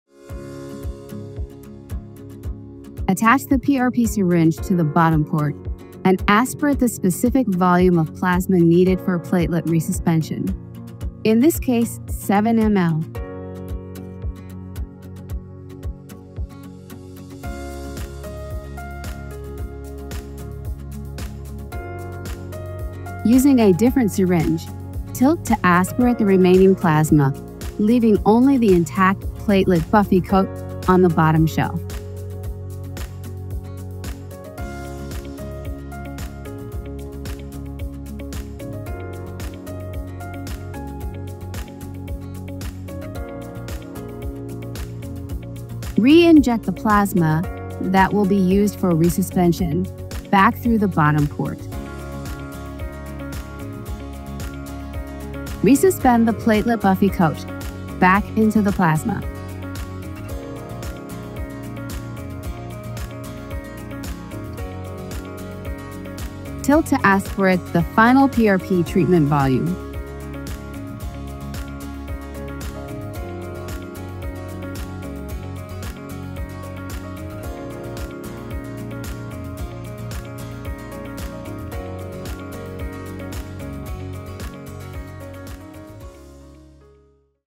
Precision Volume Technique SupraPhysiologic
Get the exact volume you need for your PRP treatment.